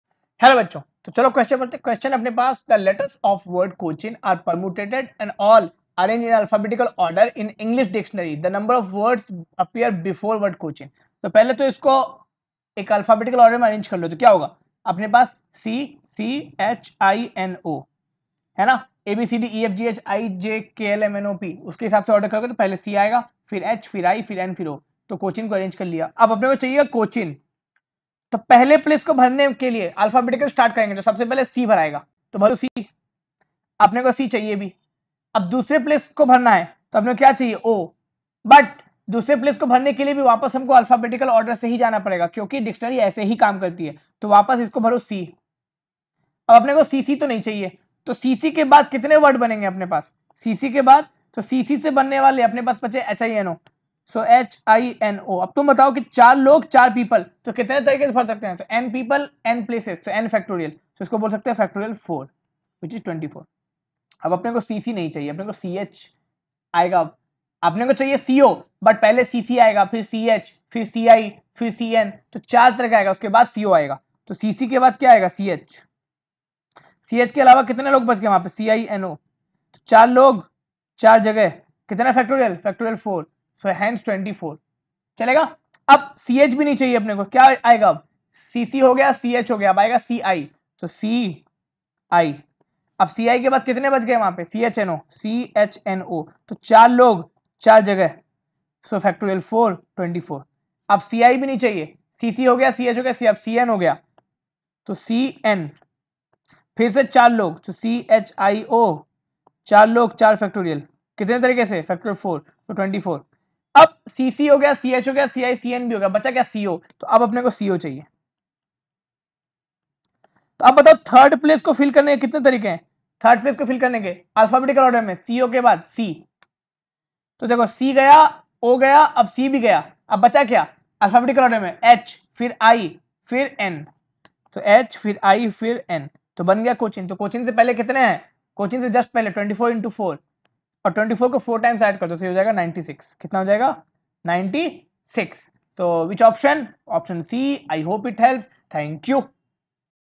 The letters of the word COCHIN are permuted and all the permutation...
The letters of the word COCHIN are permuted and all the permutations are arranged in an alphabetical order as in an English dictionary. The number of words that appear before the word COCHIN, is
(2007, 3M)
(a) 360
(b) 192